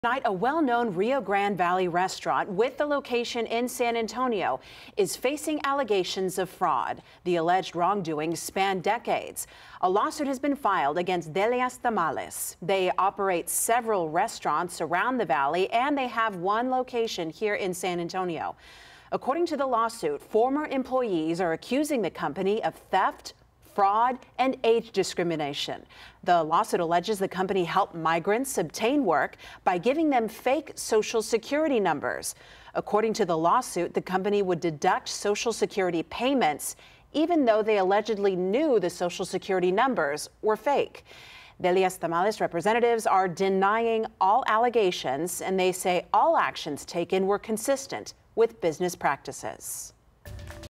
Delia's Tamales facing allegations of fraud, the alleged wrongdoings spanning decades
Former employees are accusing the company of theft, fraud and age discrimination.